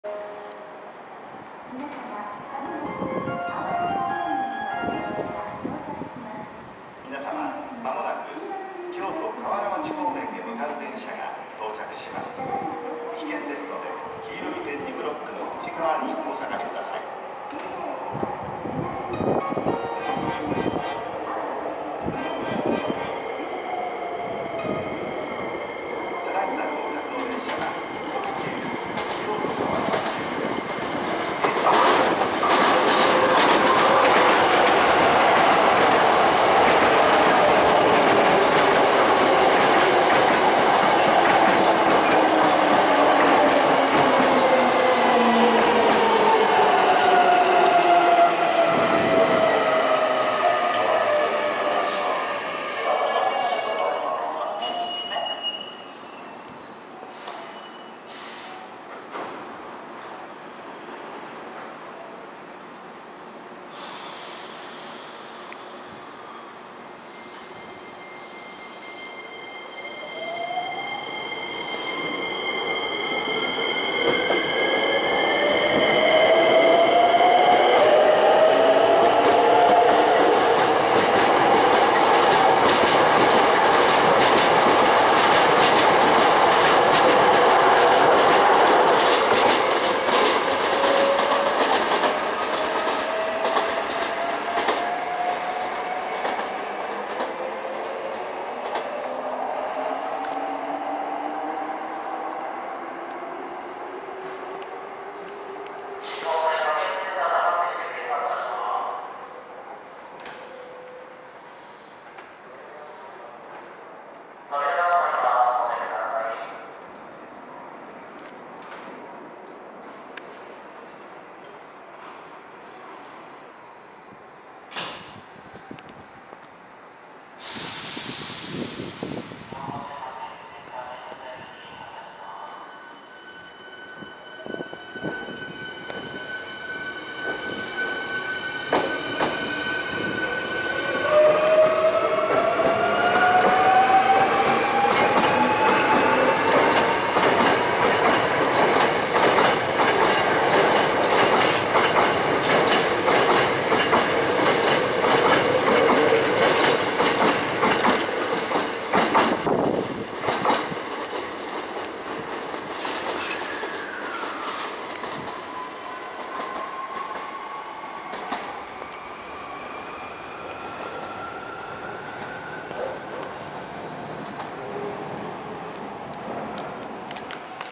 阪急 8300系 8333F+8314F GTOインバータ 準急 大阪梅田行き 9300系 9308F 特急 京都河原町行き 8300系 8300Fメモリアルトレイン 普通 高槻市行き 茨木市駅
阪急

8300系 8333F+8314F
GTOインバータ
準急 大阪梅田行き

9300系 9308F
特急 京都河原町行き

8300系 8300F
メモリアルトレイン
普通 高槻市行き

茨木市駅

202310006